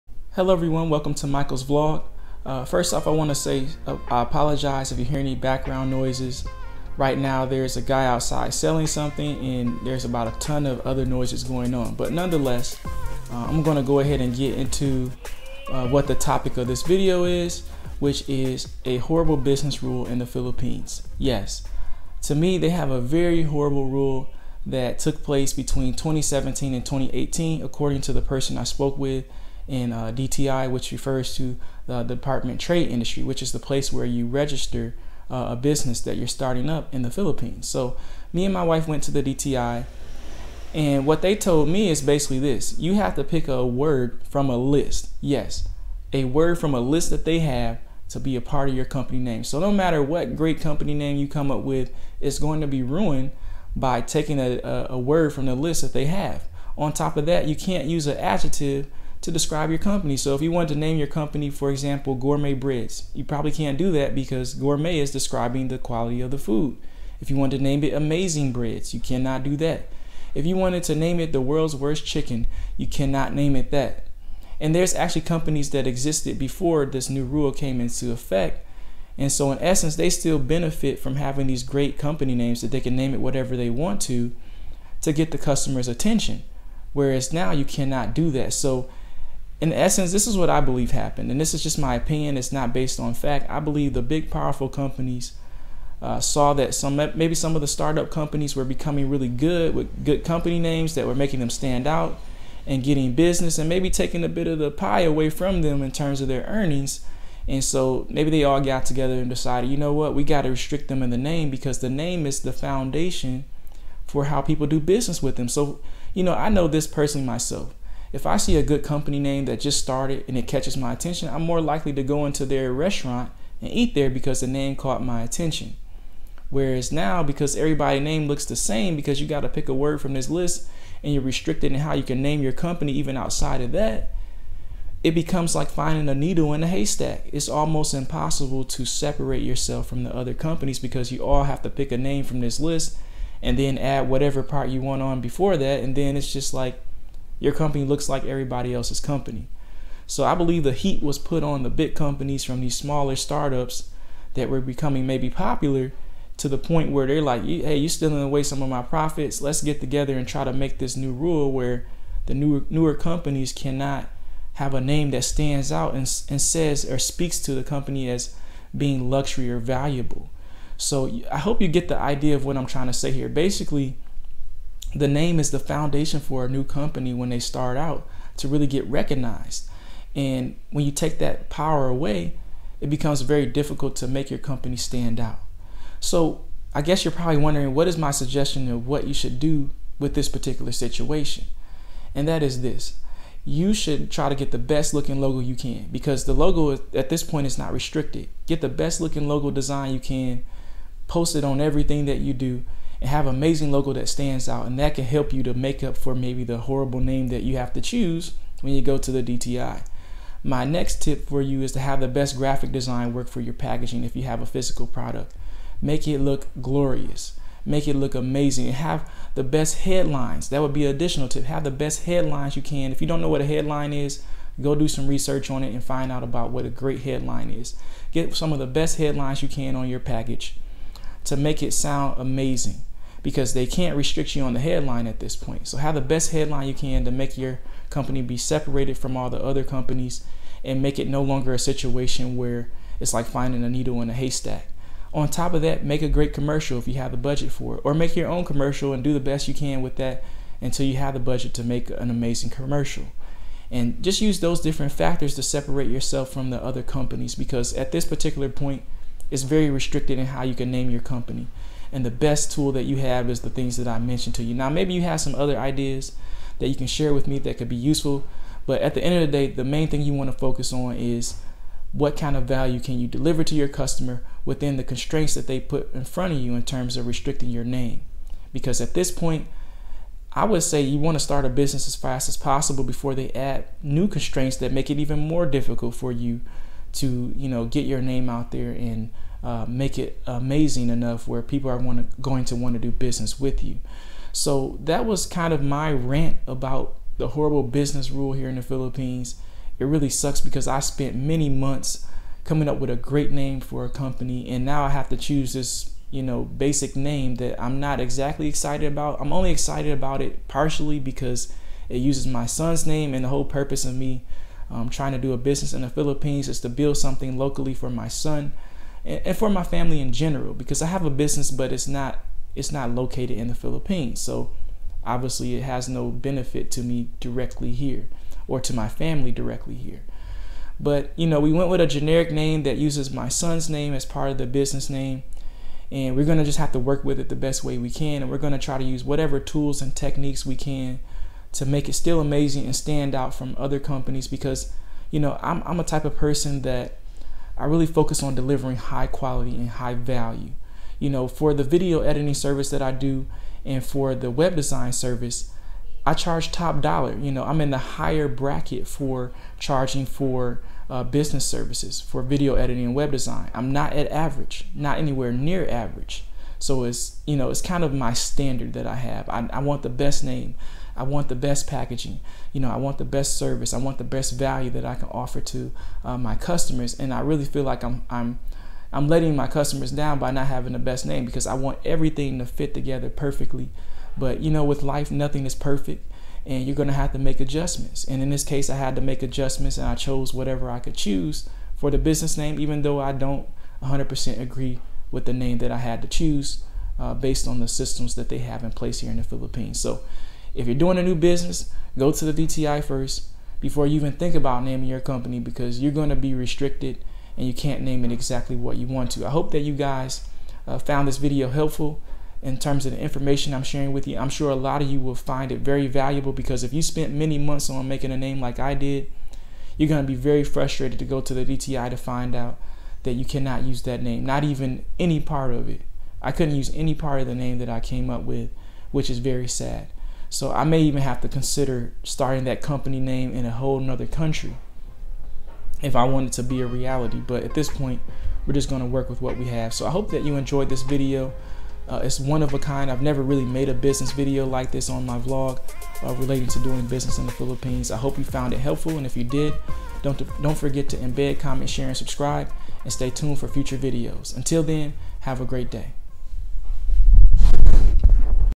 Tormenting Business Naming Rule In The Philippines Michaels Vlog 23
One major disadvantage of starting up a business in the Philippines is a new rule that requires you to structure your business name around a list of names the DTI provides on paper. Basically your business name is trapped into a structure rather you like it or not. Very little control on making your brand name stand apart from competitors. Keep in mind I am speaking in reference to Davao city I can't speak for other cities and what the requirements are.

If you enjoyed this vlog let me know what other types of videos you would like to see from the Philippines.